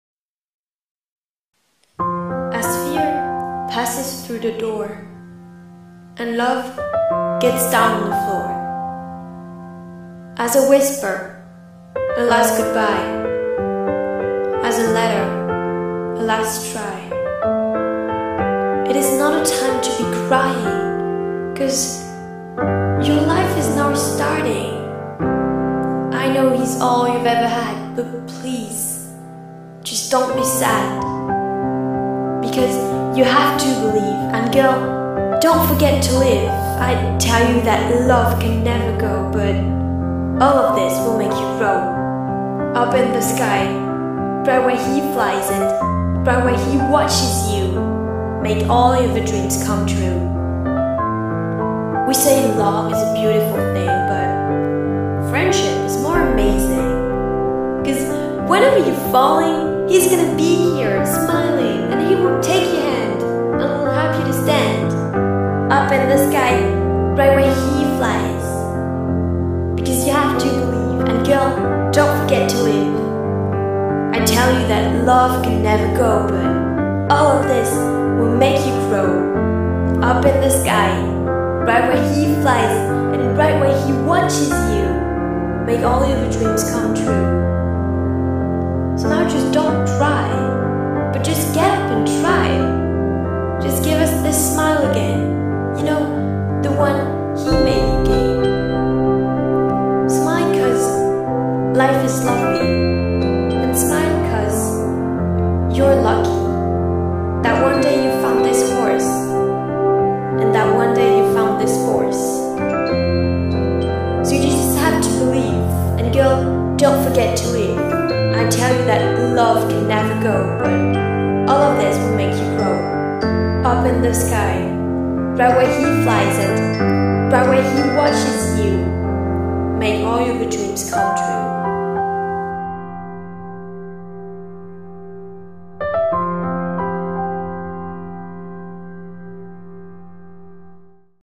You Have to Believe [Cali & Sarah]
You just have to believe, and don't forget to live.
These are my words, my poem, but not my music.